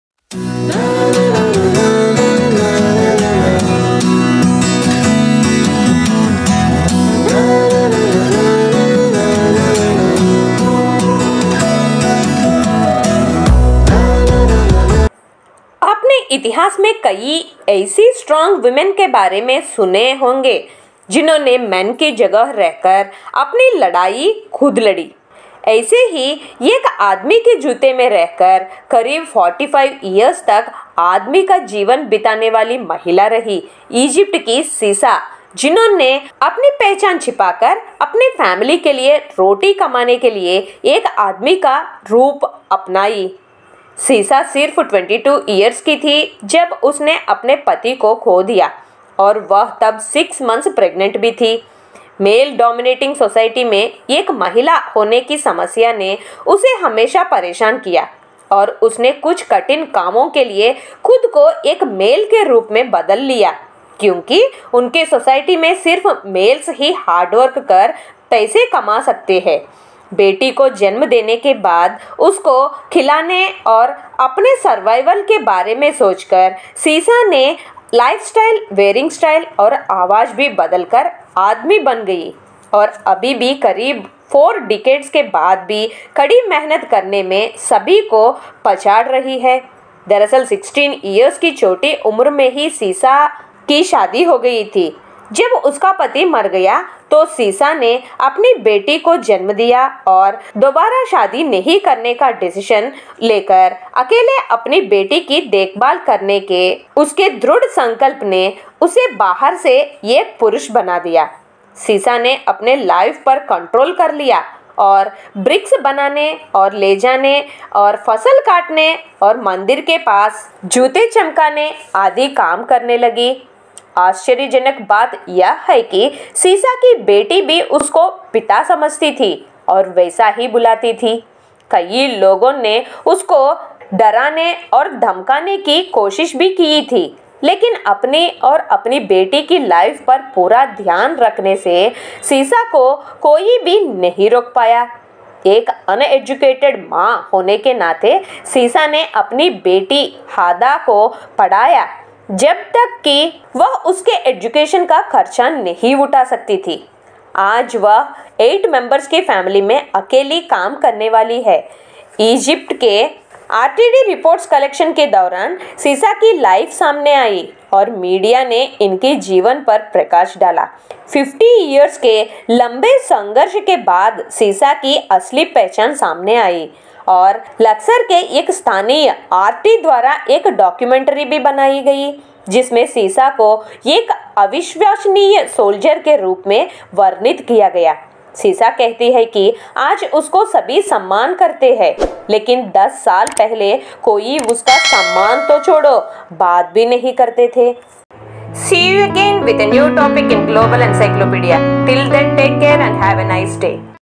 Woman Lives As Man | Global Encyclopedia
This video contains detailed information of Sessa of Egypt who lives as man since 45years.

TAGS